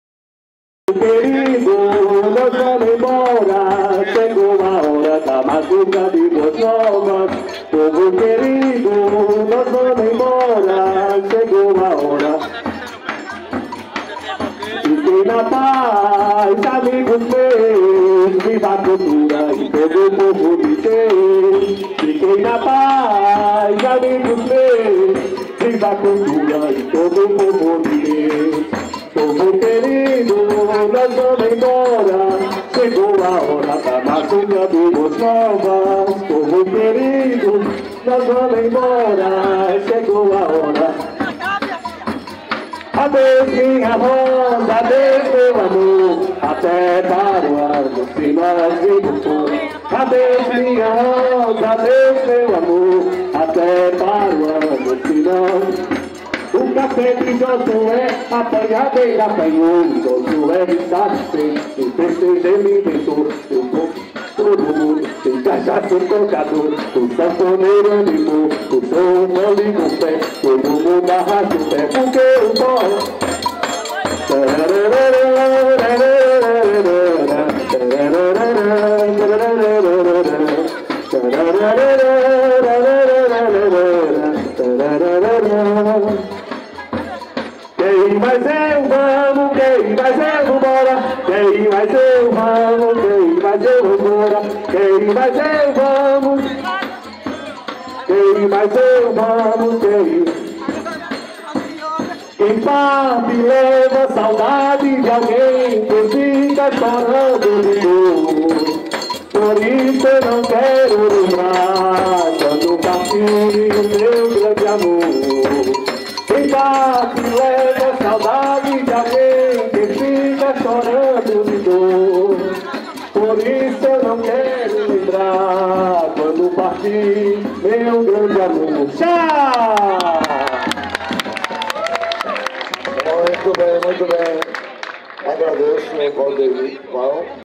Festa de São João - Praça da Cohab em Bezerros
Festa de São João - Praça da Cohab em Bezerros PE
Dia de muita alegria e diversão, agradecemos ao padre e todos da paroquia de Nossa Senhora Aparecida.